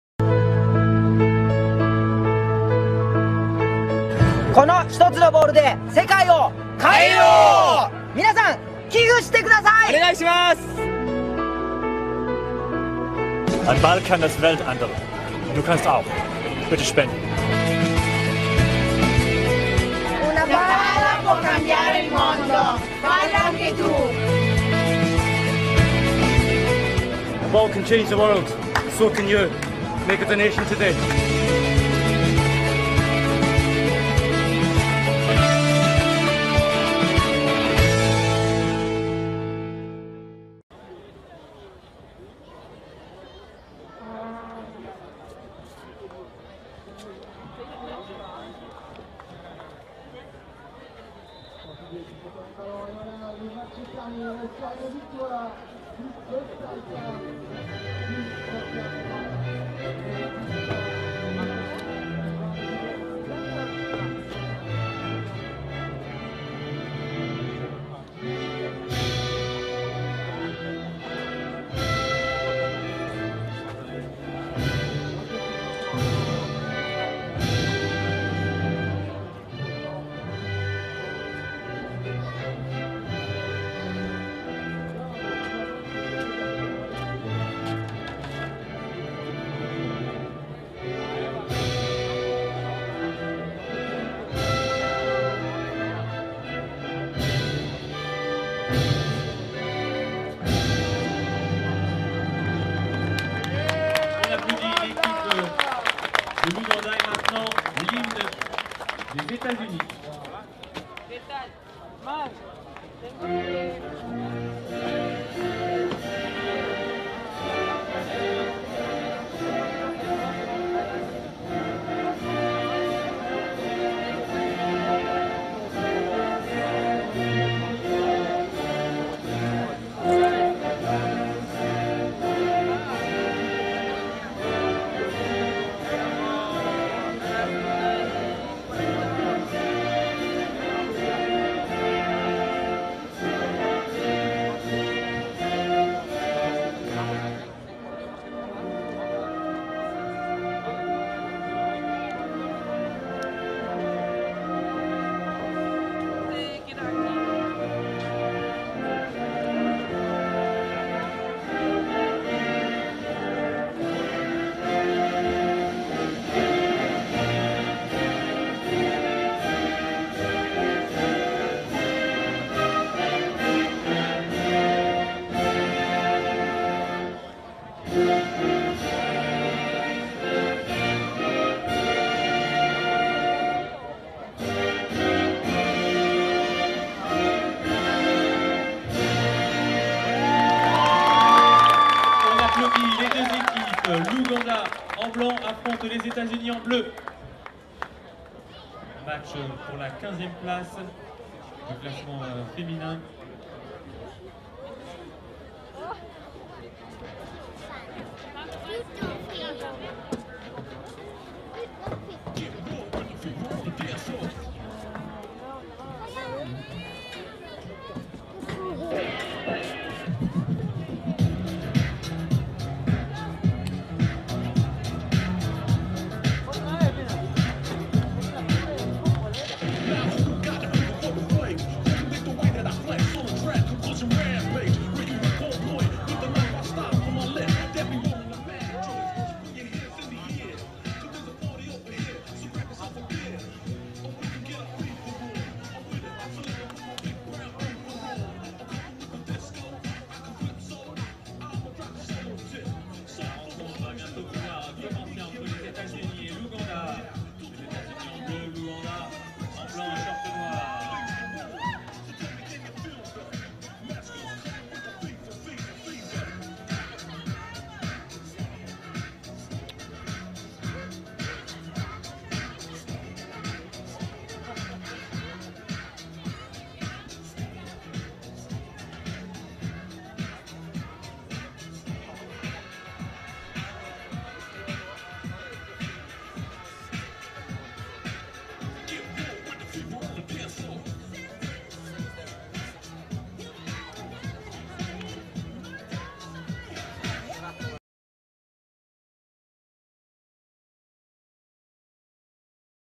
Uganda vs USA - Women's Cup 15/16th Placement - Match Clip with English Commentary
Watch a short clip of the Uganda vs USA game from the 2011 Paris Homeless World Cup.  Tell all your friends to join you in watching full matches and please donate if you enjoy.